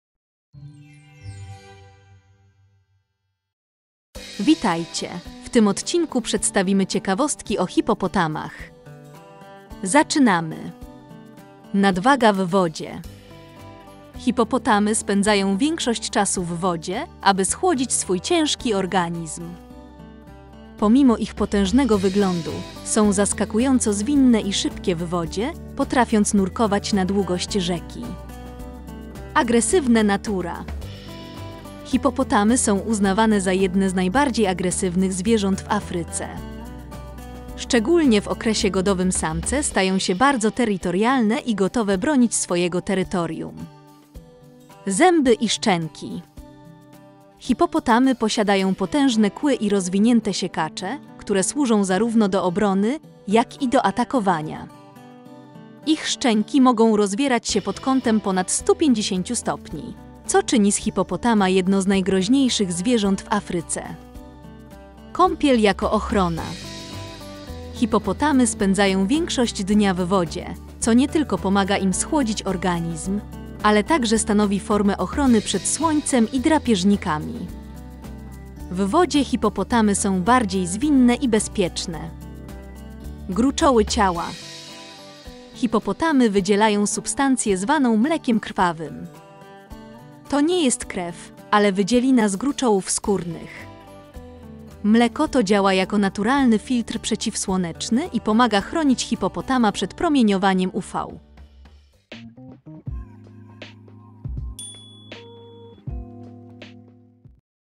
HIPPO. Ciekawostki o Hipopotamach. Film dla dzieci.
Discover my latest books! Dive into exciting stories for Kids.

Ciekawostki o Hipopotamach. Film dla dzieci.

Witajcie na naszym kanale! Przygotowaliśmy dla was film o Hipopotamach.

Nie zapomnij udostępnic innym 👨‍👩‍👦 i zostawić 𝐋𝐀𝐉𝐊𝐀 👍